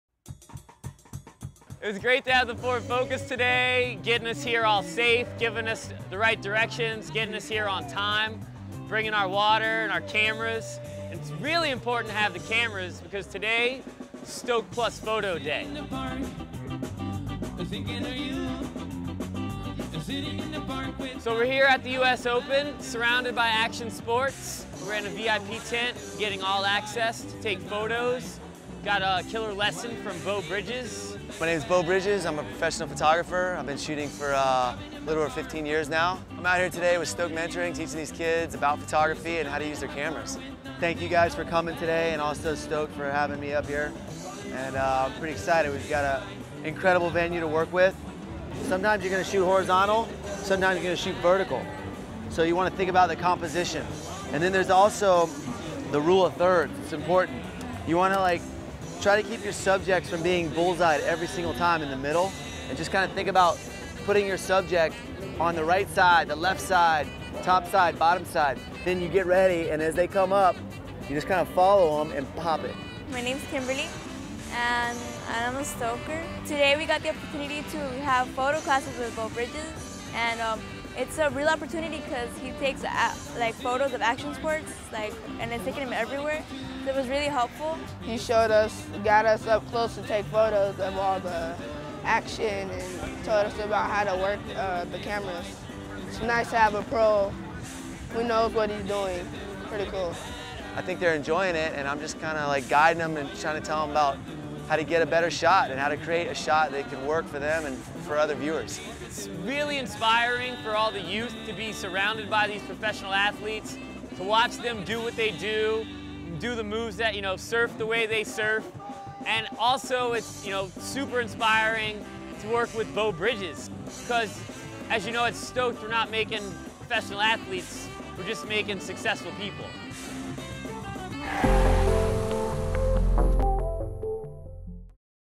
Stoked Mentoring - Episode 4
Charlie and the Stokers team up with world renowned action sports photographer, Bo Bridges, for Stoked Plus Photography at the 2011 Nike US Open of Surfing in Huntington Beach.